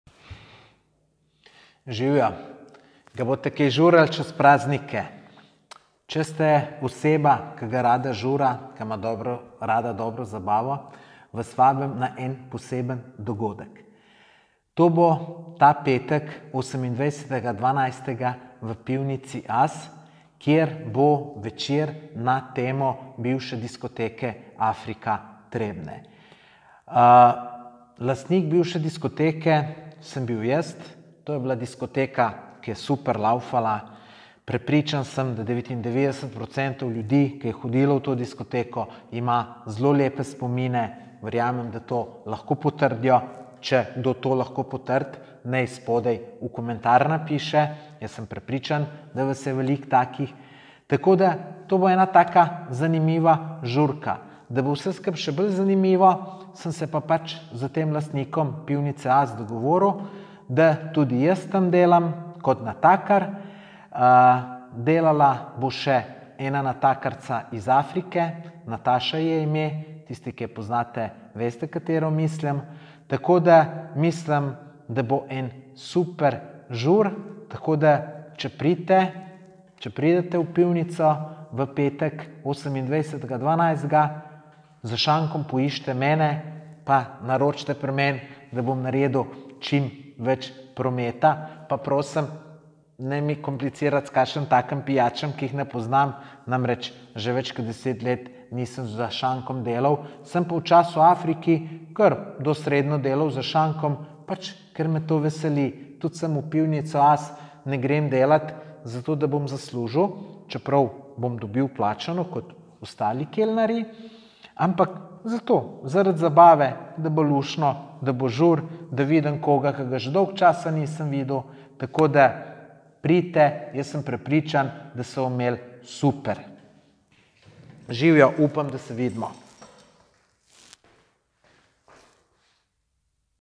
Africa Reunion / Back In Time - Pivnica AS / Petek 28.12.2018
Prideš na zabavo, kjer se bomo vrnili v čase diskoteke Africa?
Na dogodku bova delala za šankom dva, ki sva delala tudi v Africi. Poleg mene je to še Nataša. Klikni če si zainteresiran za dogodek.